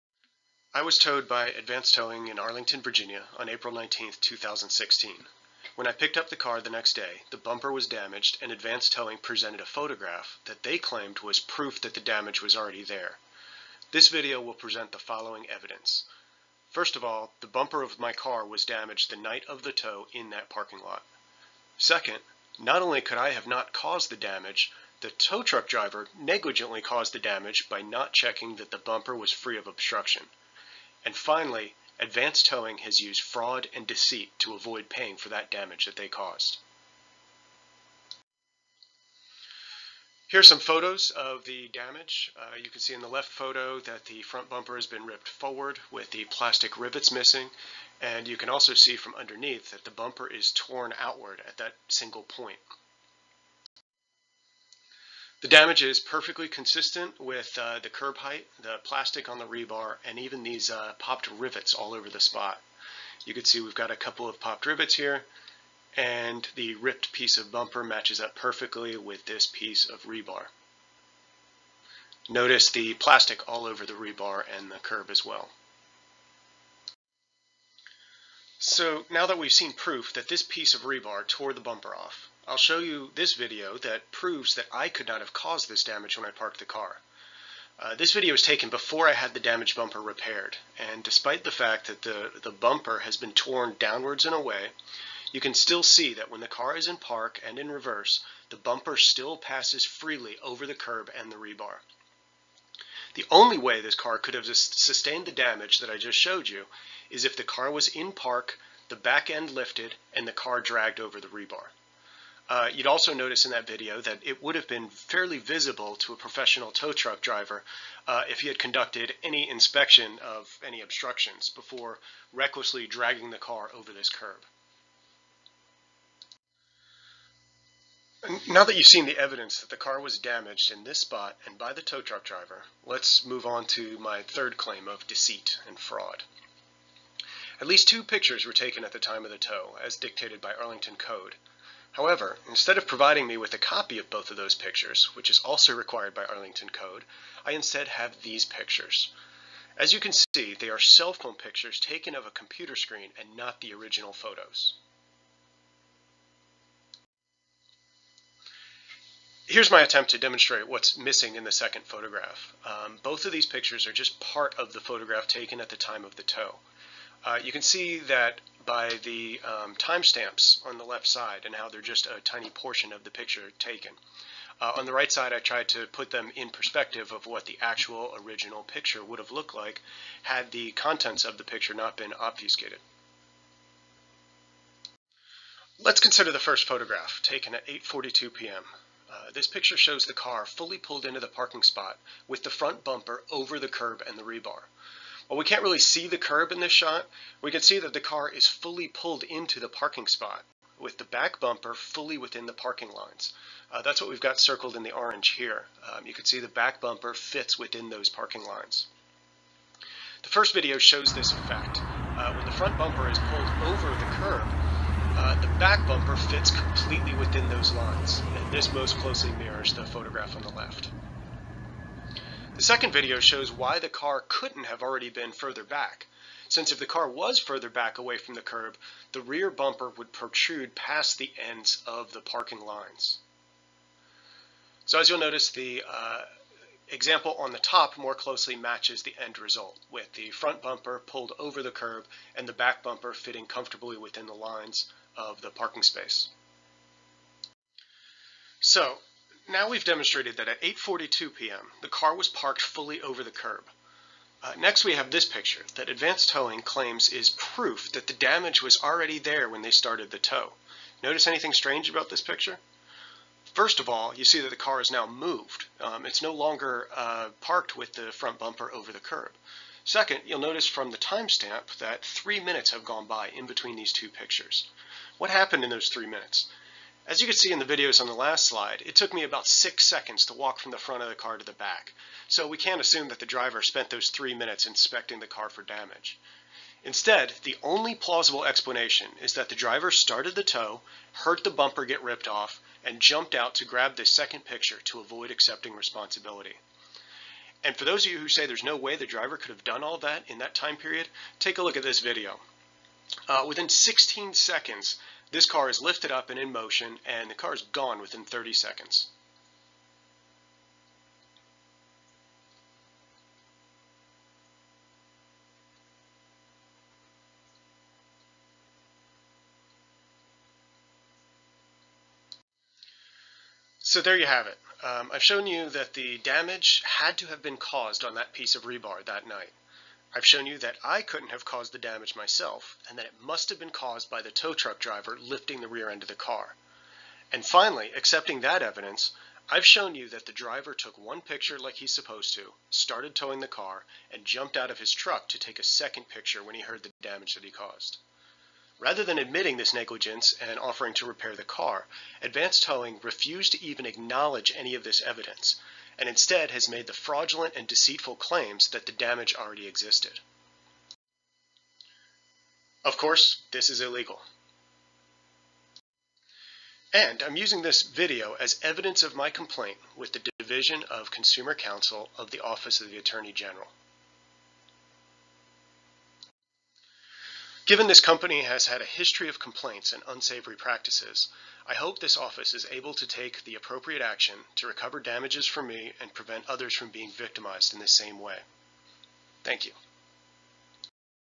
Advanced Towing Fraud
Presentation to the Division of Consumer Counsel of the Office of the Attorney General of Virginia.  Documentation of deceitful and fraudulent practices of Advanced Towing Company in Arlington, Virigina.